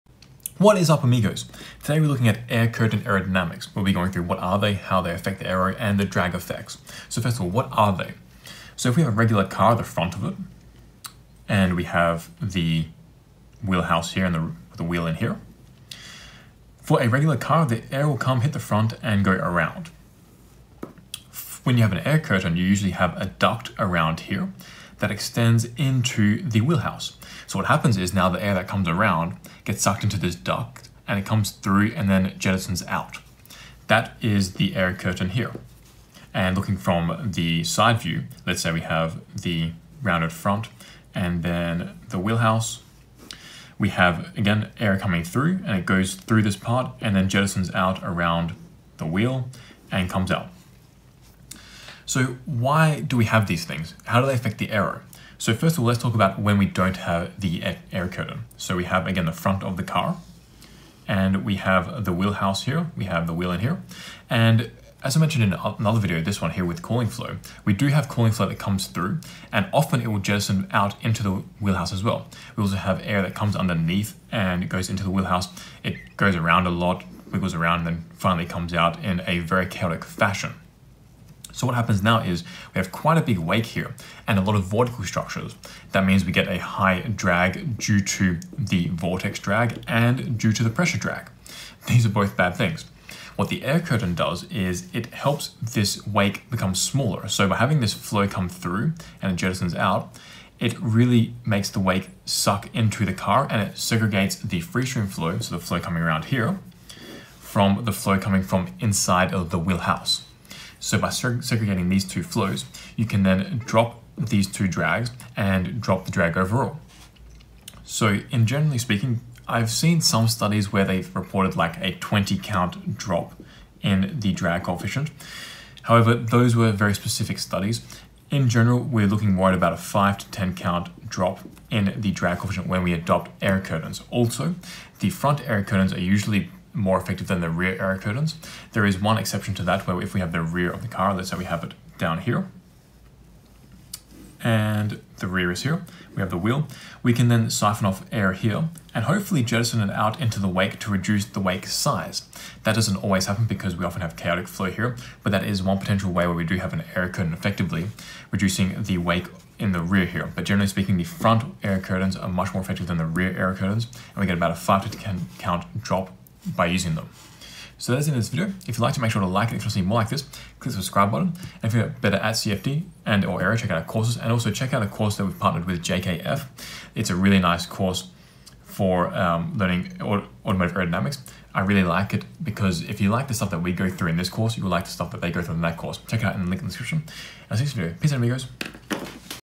{Automotive Aero #22} Air Curtains Explained
Air curtains have become widely adopted. How do they affect a car's aerodynamics? How do they affect a wheel's aerodynamics, and the overall drag coefficient?

That's what this video explains.